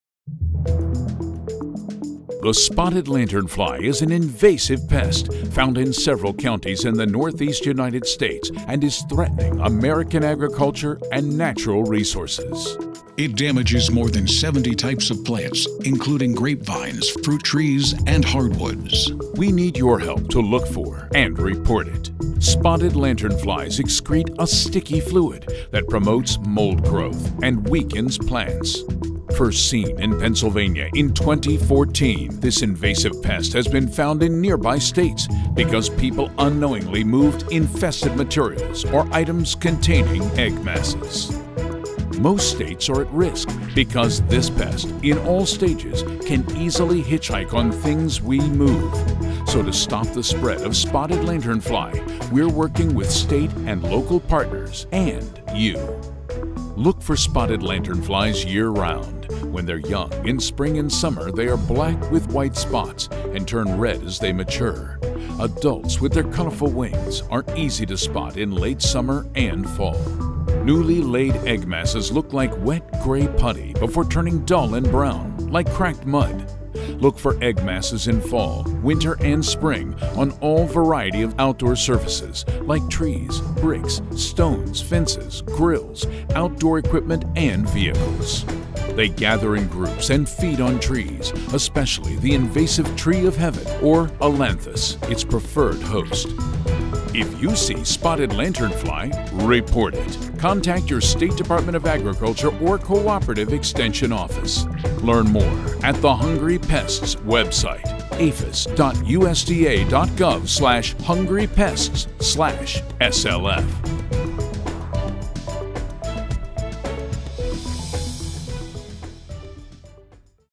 Look For and Report Spotted Lanternfly
The spotted lanternfly is an invasive pest found in several counties in the Northeast United States, and is threatening American agriculture and natural resources. It damages more than 70 types of plants, including grape vines, fruit trees, and hardwoods.  We need your help to look for and report it!

Spotted Lanternflies excrete a sticky fluid that promotes mold growth and weakens plants. First seen in Pennsylvania in 2014, this invasive pest has been found in nearby States because people unknowingly moved infested material or items containing egg masses.

Most states are at risk because all stages of this pest can easily hitchhike on things we move. So to stop the spread of spotted lanternfly, we’re working with state and local partners – and YOU!

Look for spotted lanternflies year round. When they’re young, in spring and summer, they are black with white spots, and turn red as they mature. Adults with their colorful wings are easy to spot in late summer and fall. Newly laid egg masses look like wet grey putty, before turning dull and brown, like cracked mud. Look for egg masses in fall, winter, and spring on all variety of outdoor surfaces, like trees, bricks, stones, fences, grills, outdoor equipment, and vehicles.

They gather in groups and feed on trees, especially the invasive tree of heaven, or Ailanthus, its preferred host.
If you see spotted lanternfly, report it! Contact your State Department of Agriculture or Cooperative Extension Office.

A message from the USDA.